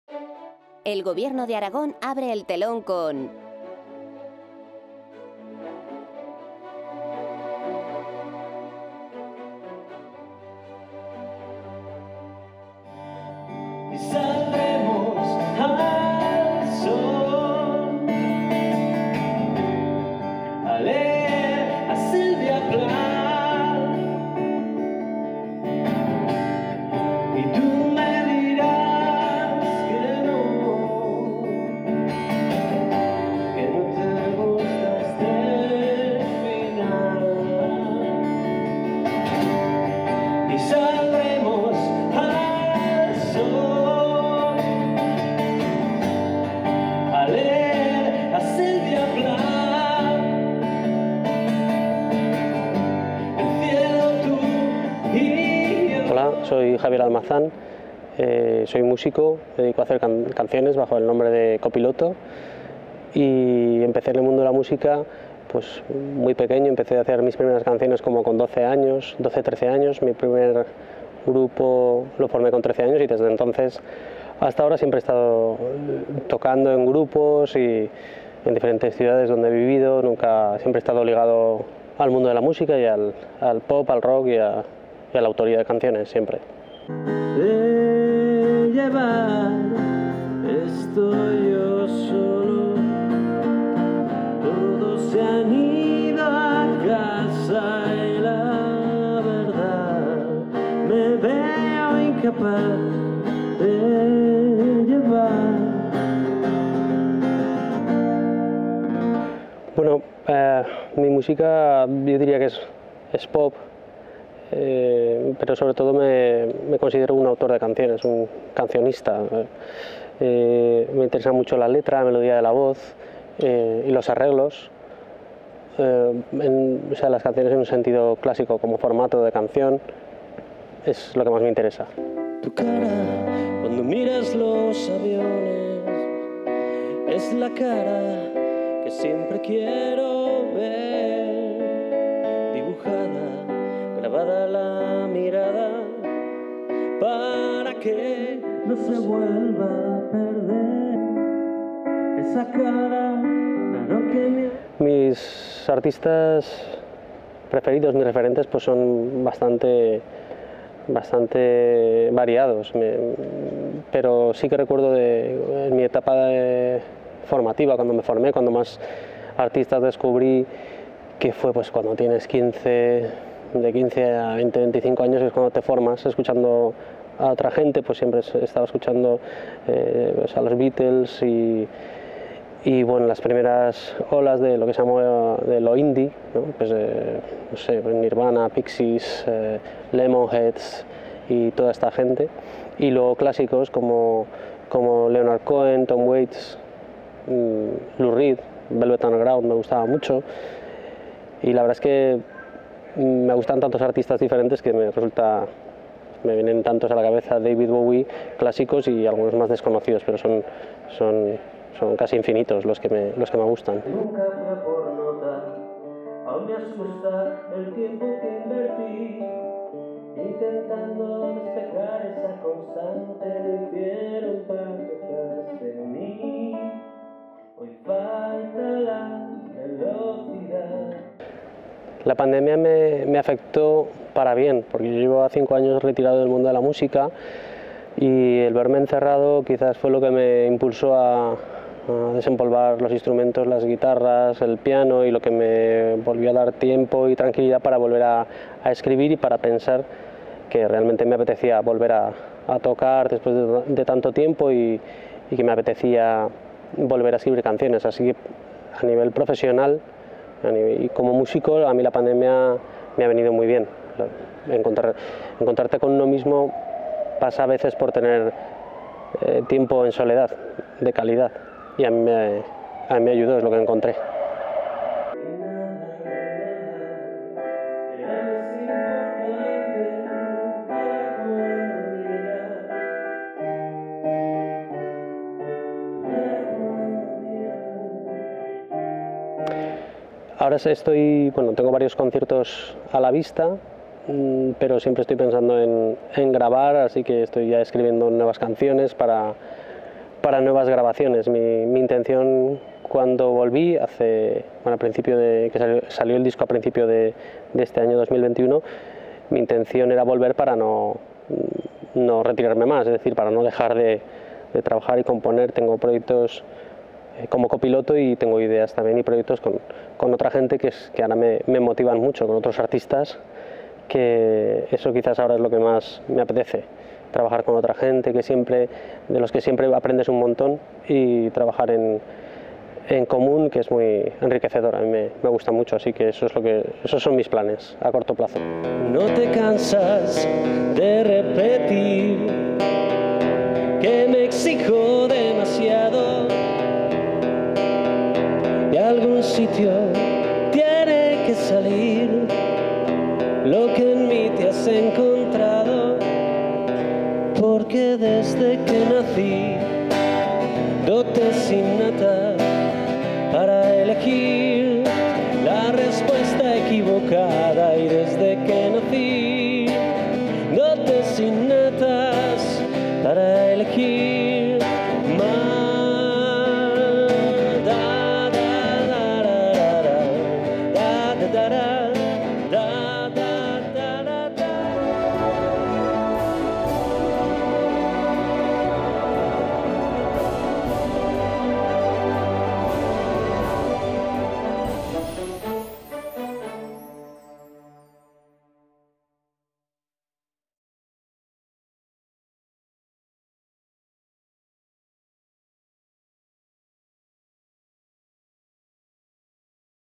Entrevista a Copiloto / Concierto en Múver 2021
Concierto de Copiloto con Aragón Musical dentro de Múver 2021 promovido por el Gobierno de Aragón, en el IAACC Pablo Serrano el pasado 21 de agosto.